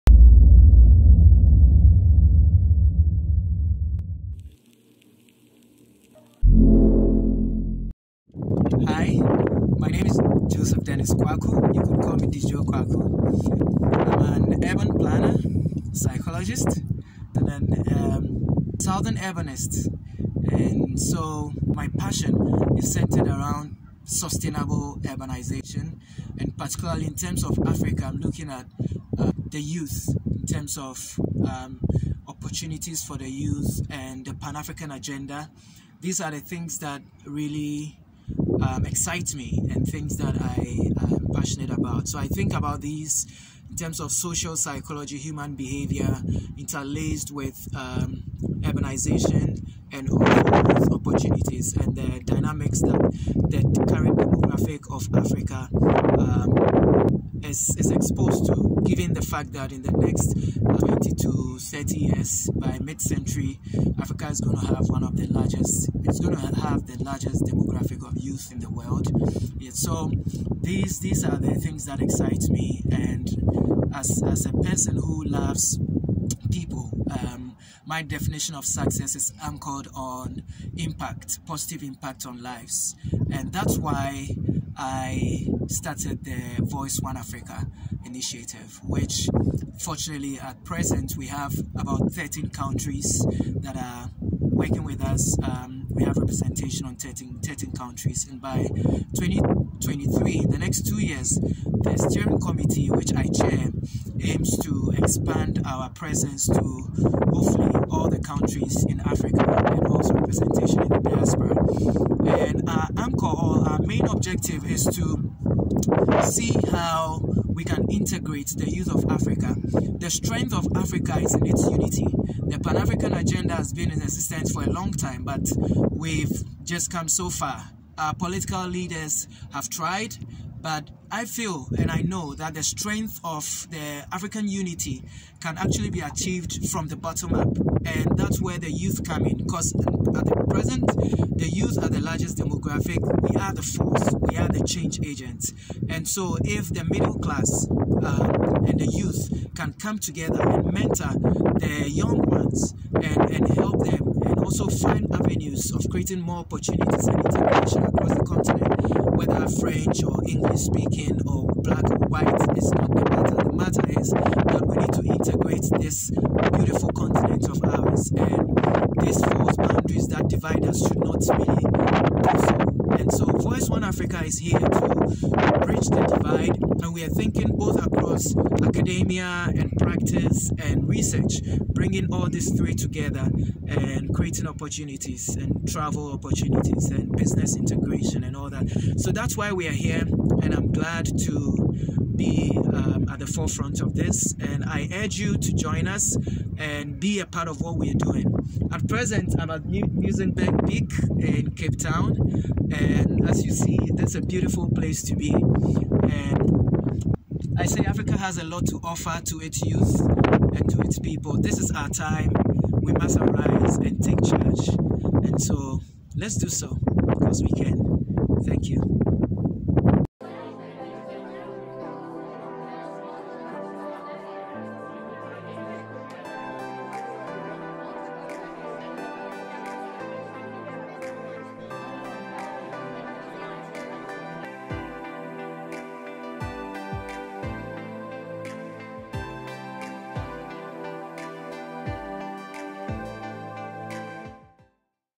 Driven by the passion for positive impact in Africa: De Joe Quarcoo, Visionary & Chairman
With a prior background in Social Psychology, Joseph Dennis Nii Noi Quarcoo, also known as @DeJoeQuarcoo changed careers into the urban sustainability sector. The visionary and the Chair of the Founding Steering Committee of Voice1Africa gives a little insight into his life and thoughts from Muizenberg peak in Cape Town, South Africa.